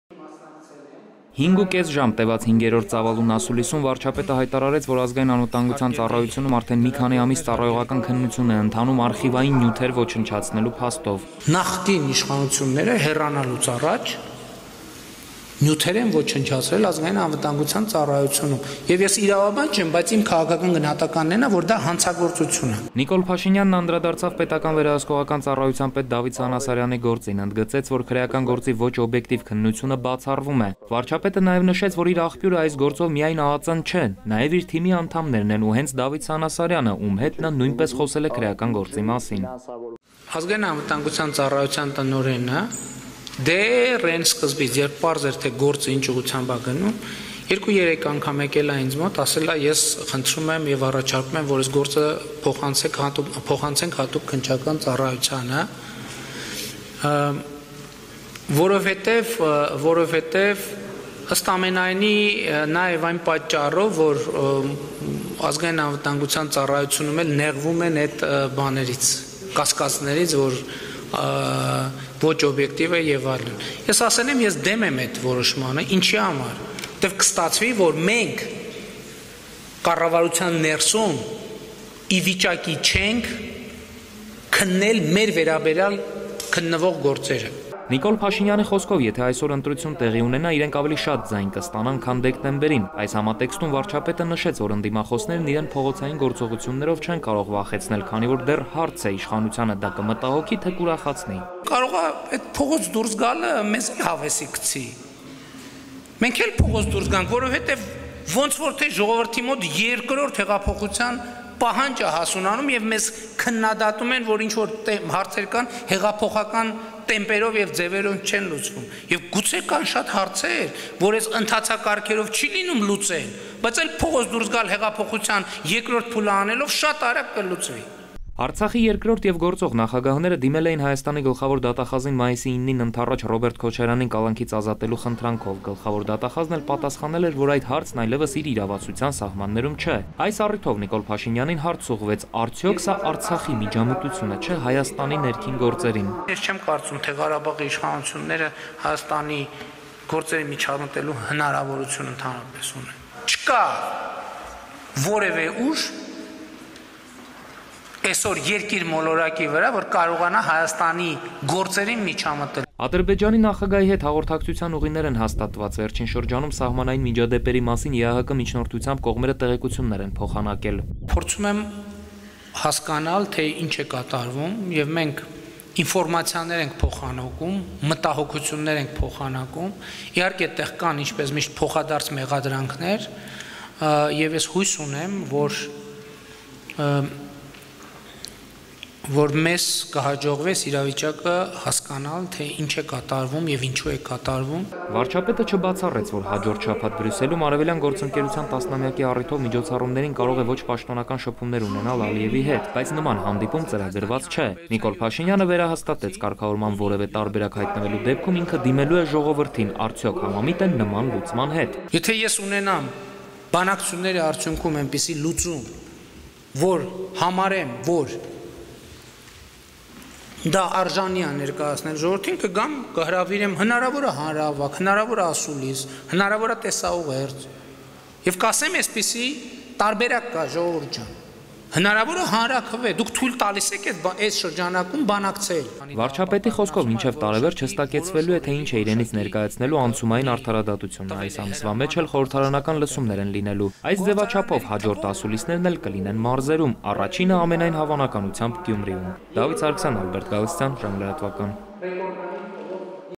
Վարչապետ. ԱԱԾ-ում ծառայողական քննություն է ընթանում ոչնչացված նյութերի գործով
5 ու կես ժամ տևած 5-րդ ծավալուն ասուլիսում վարչապետը հայտարարել է, որ Ազգային անվտանգության ծառայությունում արդեն մի քանի ամիս ծառայողական քննություն է ընթանում արխիվային նյութեր ոչնչացնելու փաստով: